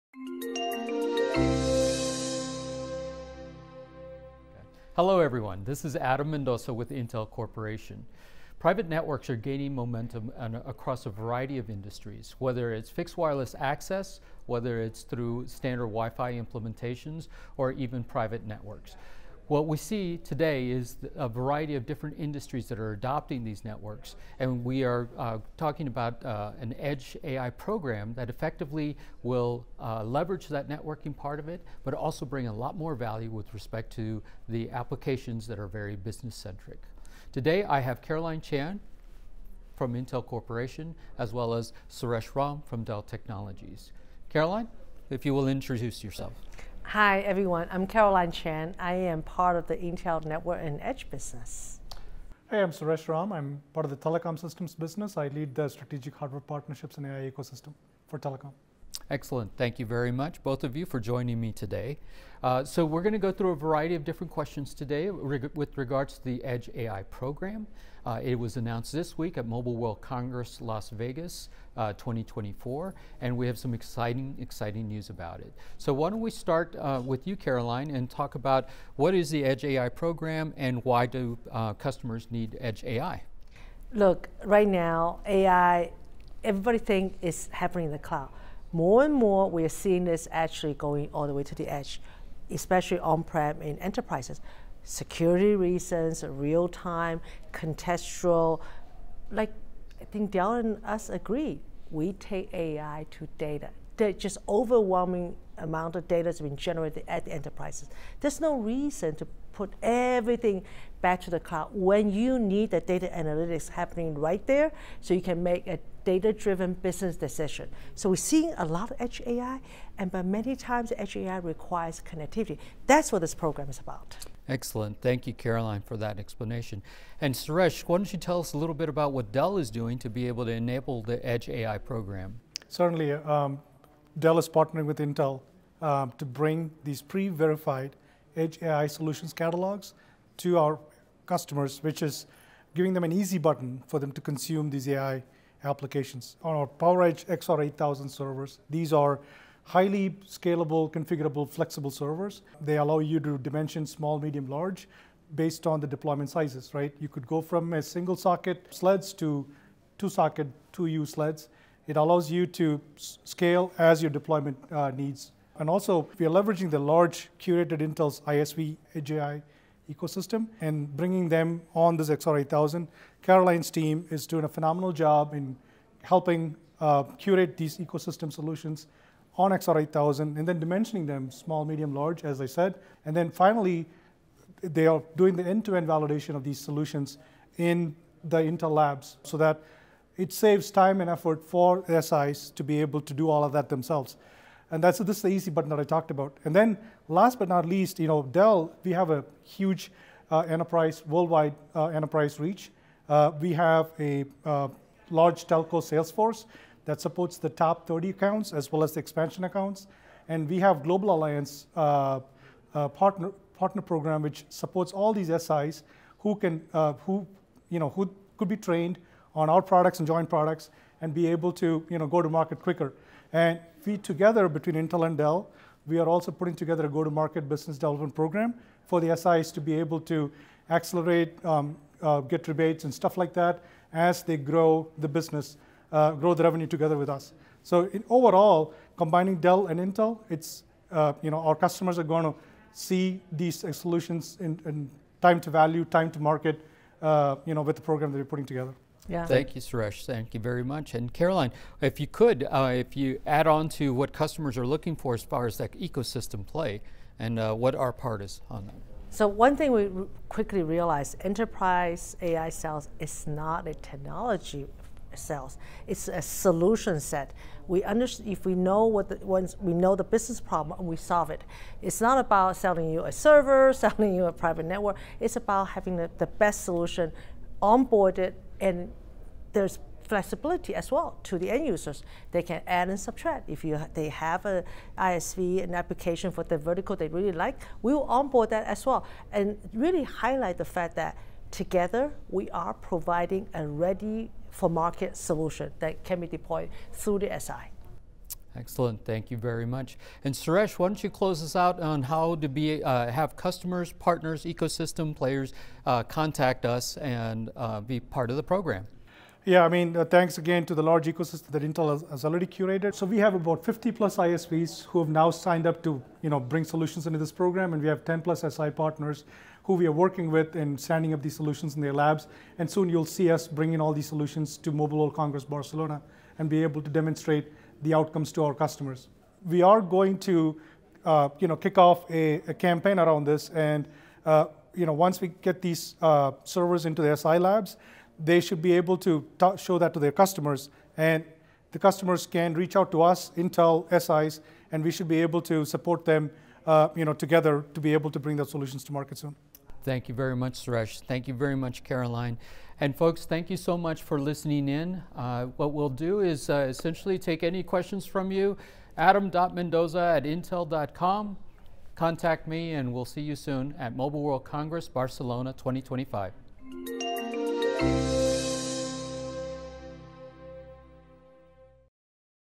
Dell Technologies, Intel combining AI and private networking at the enterprise edge
Digital transformation is a big and nebulous area but, at its core, there are some key pieces that need to be aligned to harness technologies in service of improved business outcomes and performance—artificial intelligence (AI), applications, connectivity and infrastructure. And bundling these technologies into a package that’s easy for enterprises to consume is the focus of the Edge AI Program co-developed by Dell Technologies and Intel. This interview, filmed during Mobile World Congress in Las Vegas, features Dell's Suresh Raam and Caroline Chan and Adam Mendoza of Intel.